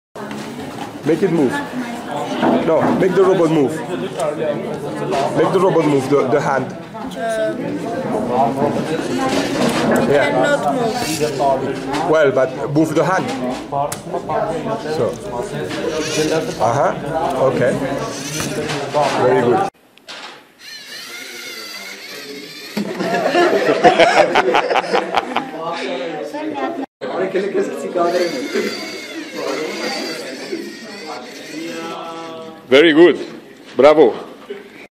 130208 TUMO Robotics presentation 01
One of the rescue robots that follow a line, negotiate gaps, climb a ramp, turn at intersections and grab a victim, designed by the youngest students of the workshop!